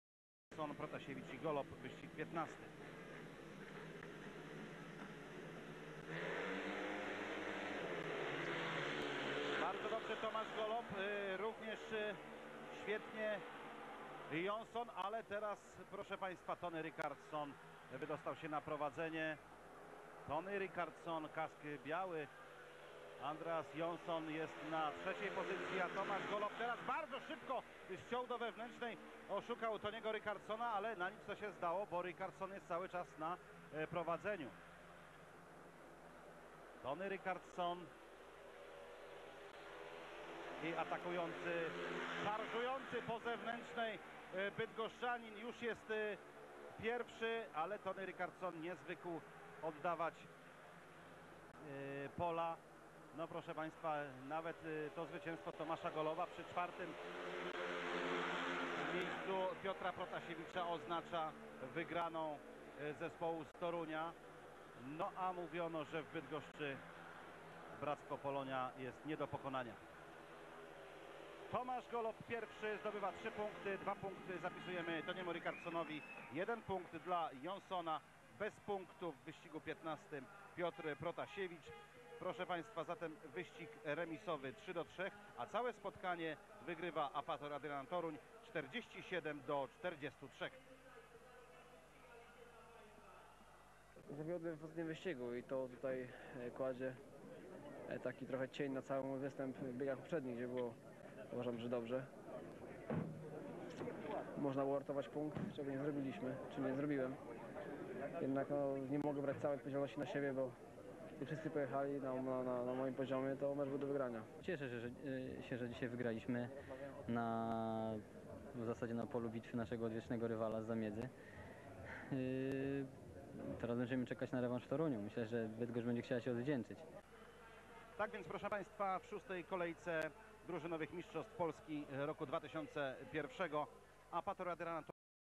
Gollob vs Rickardsson Bydgoszcz 2001 DMP (Polonia - Apator 15 bieg)
15 bieg meczu rundy zasadniczej DMP 2001.